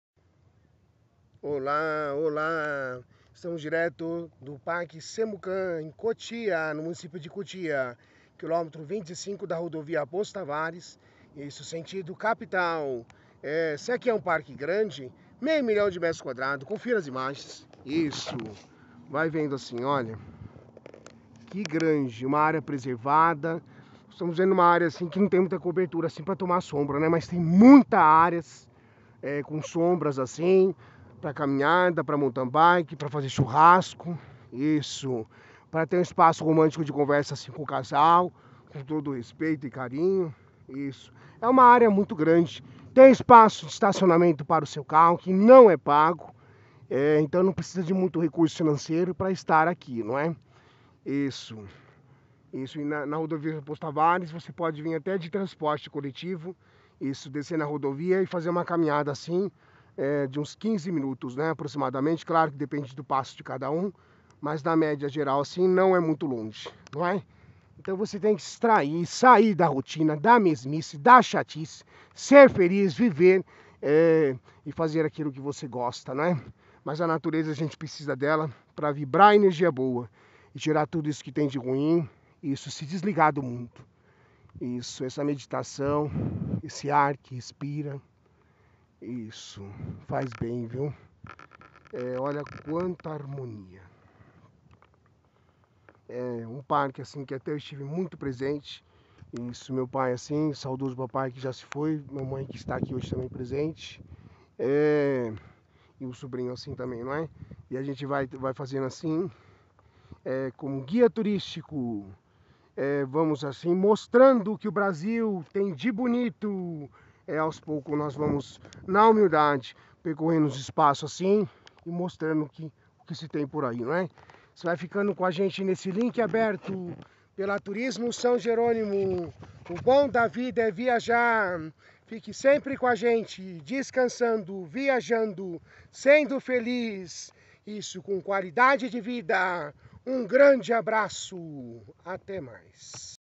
Qualidade de vida: natureza e espaços para se desligar do mundo.
Guia Turístico. Turismo São Jerônimo. Parque CEMUCAM, Cotia-SP. Com Sr. Ednilson Lourenço de Moraes.